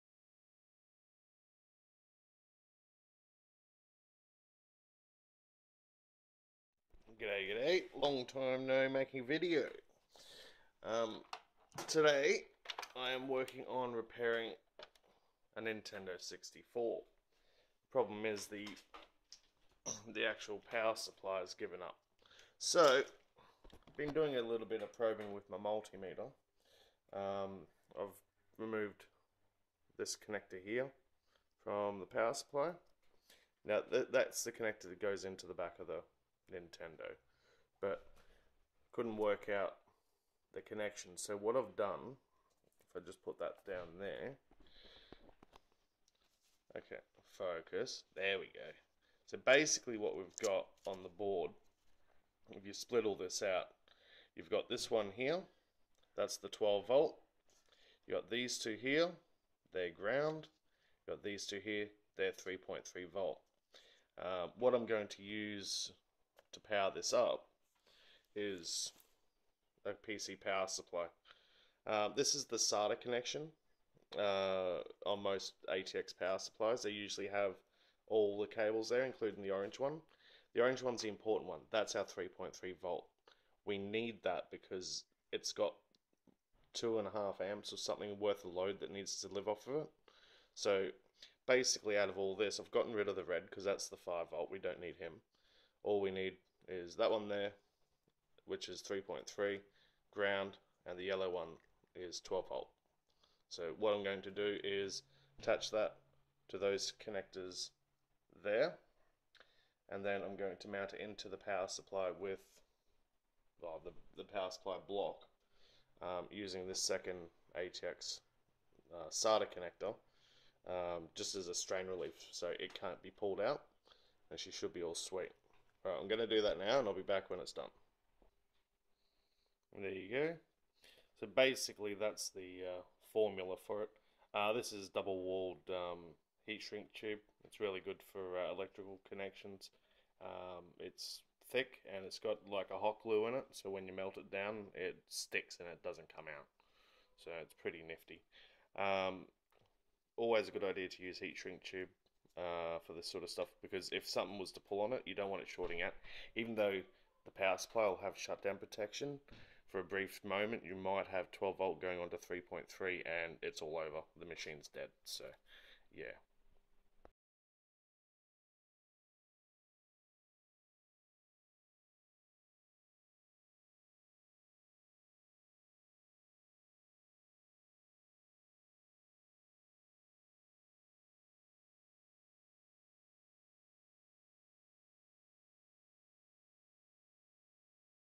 Nintendo 64 power supply pinout and diy repair
Repairing a Nintendo 64 power supply with a pc psu